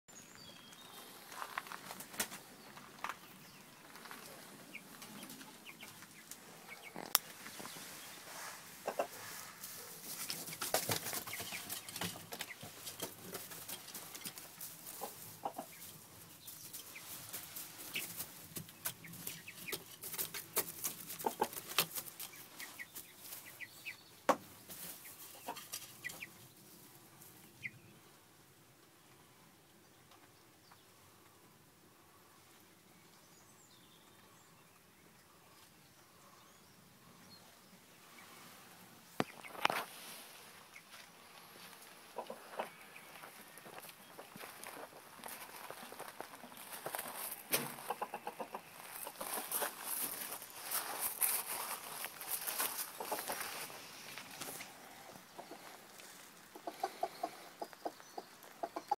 Installation d'une nurserie pour poussins
Afin de gagner de la place dans le garage, j'ai instalé la nurserie dans le poulailler.
Installée le 07 avril 2013